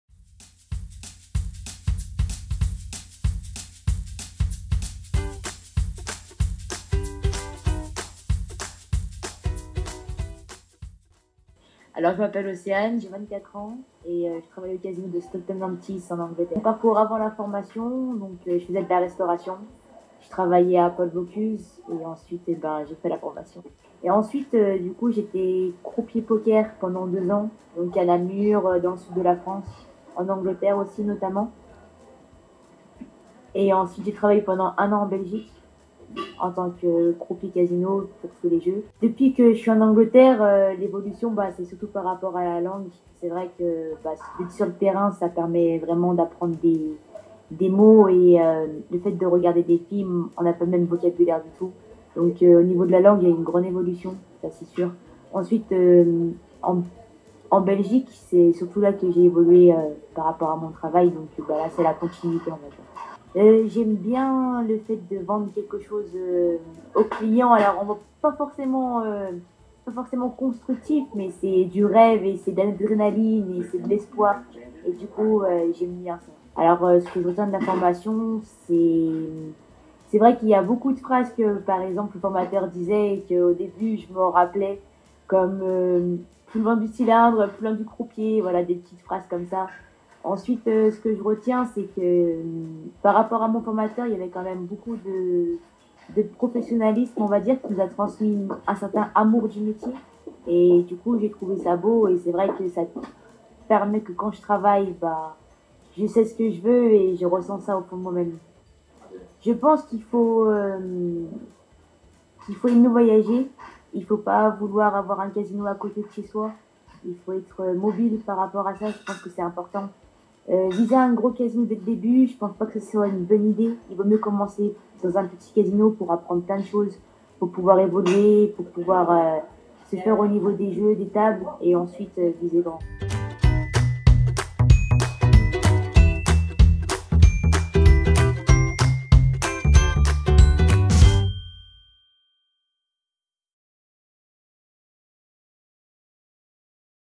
Portrait d'ancien Cérus Casino Academy : Océane MARGUERON, Croupier en Angleterre
Océane MARGUERON, croupière en Angleterre ancien élève à Lyon , répond à nos 5 questions.
1- Parcours avant la Formation
2- Parcours après la formation
3- Ce que tu aimes dans le métier de croupier
4- Ce que tu retiens de la formation
5- un conseil à donner